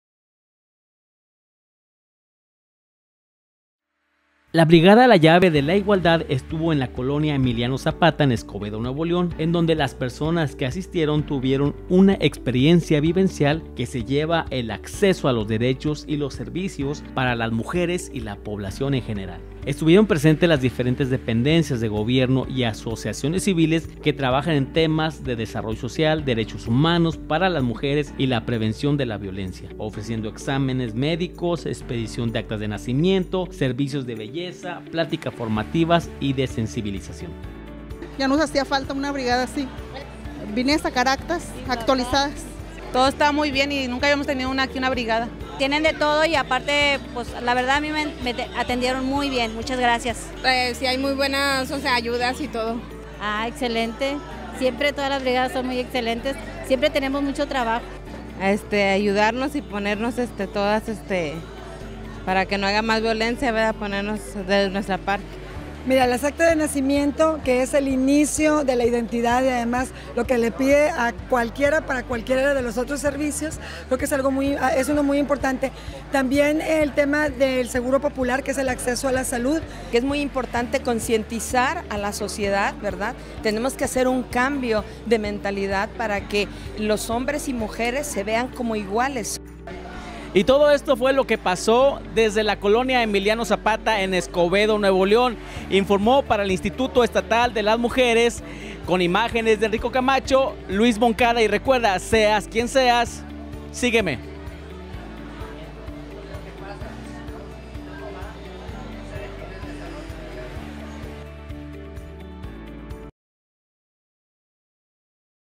Yamilett Orduña en la Brigada "La llave de la Igualdad" . 24AGOSTO
Yamilett Orduña Saide, Presidenta del Instituto Estatal de las Mujeres y Rosa Fierro Campos, Directora del Instituto Municipal de Escobedo en la Brigada "La llave de la Igualdad" en el Municipio de Escobedo NL.
24 AGOSTO 2017